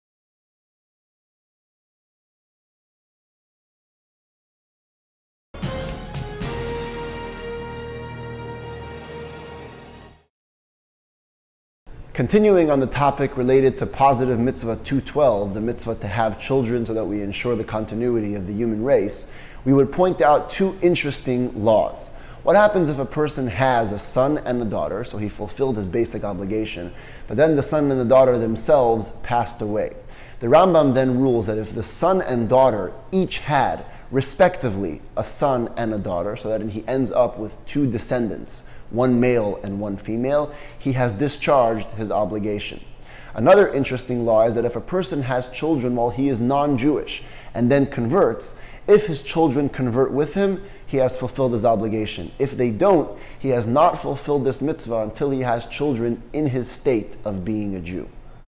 Do non-Jewish children fulfill?
Mitzvah Minute Day 77

Join Rabbi Yossi Lipskier for 60 seconds a day, on the 39th journey through the 613 Mitzvos of the Torah.

A project of Chabad of Sherman Oaks